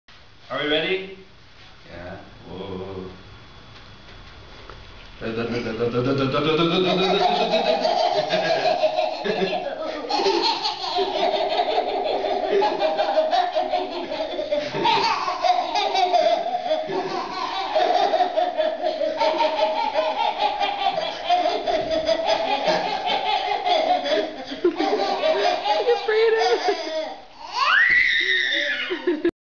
Josh and boys in tockletown 2
josh tickles rowna and brayden